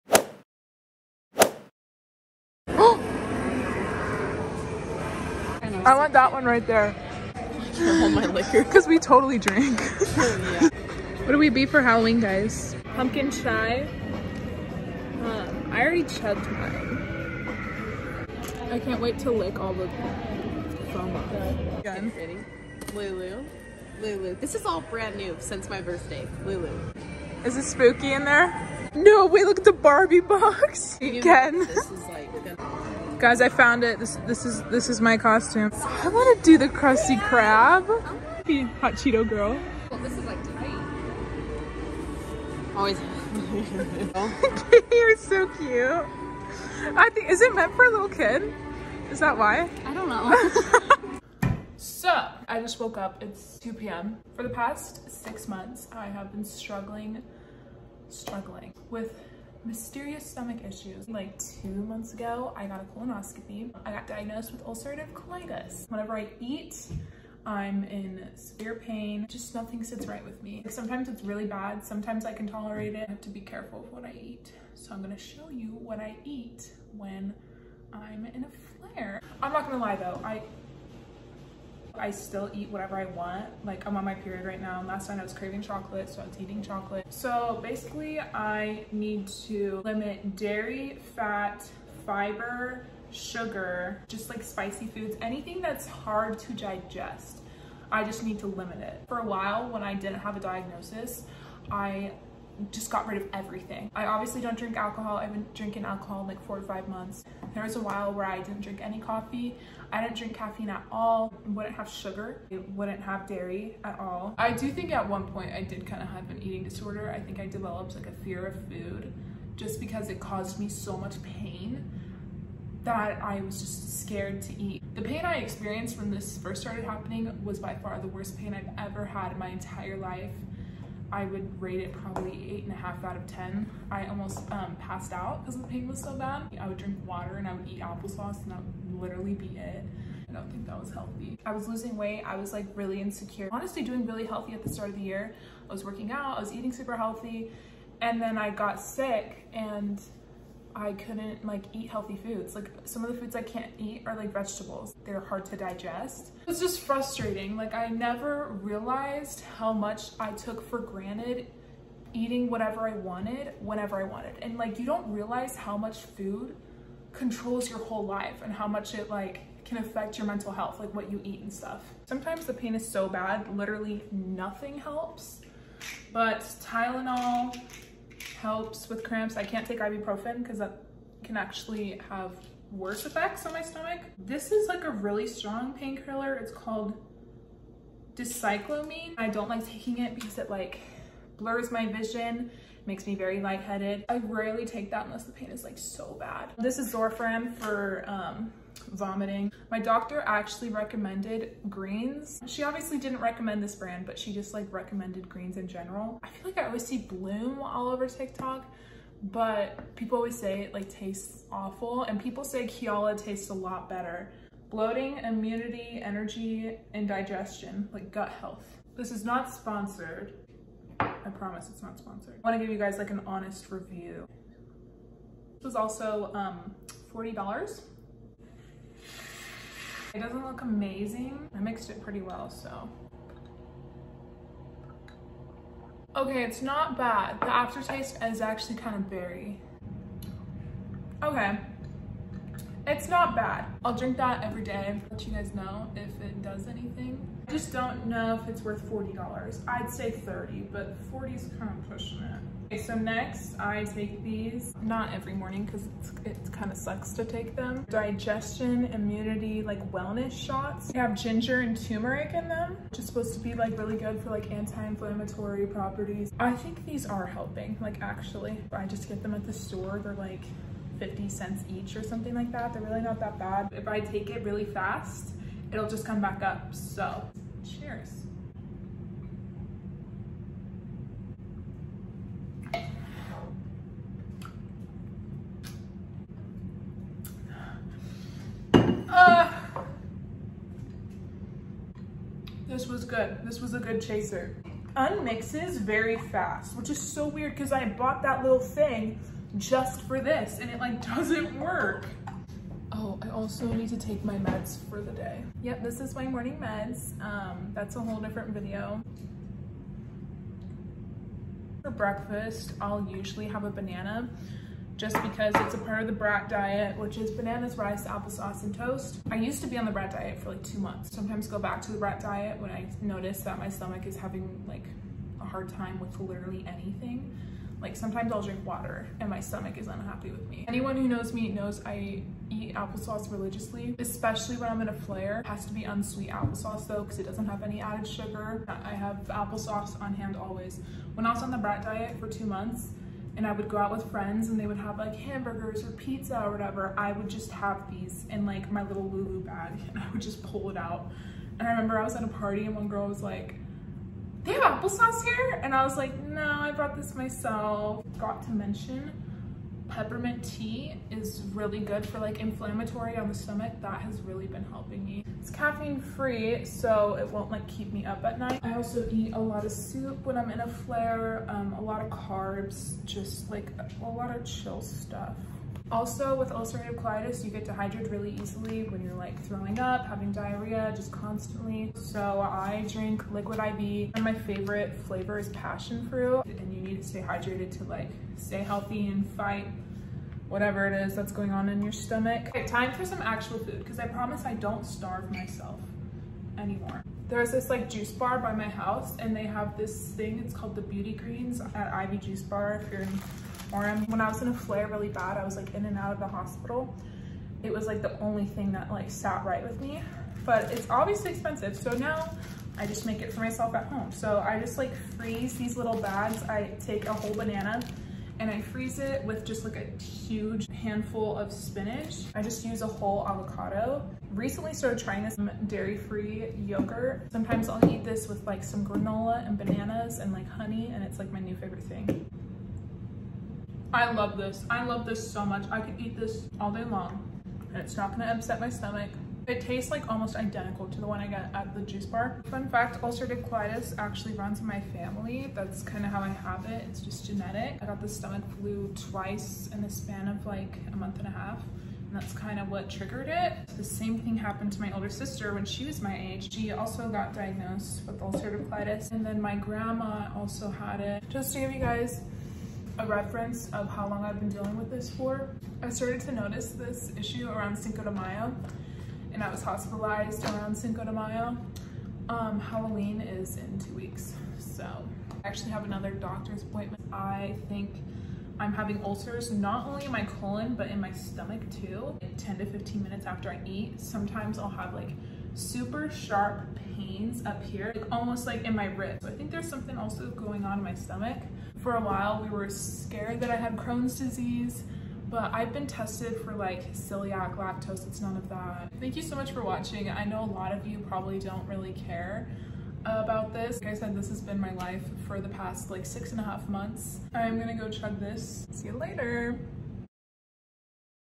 what I eat in a day with ulcerative colitis + honest kiala greens review
edit: I recently got diagnosed with cholelithiasis (gall stones)
thank you for listening to my health journey :-)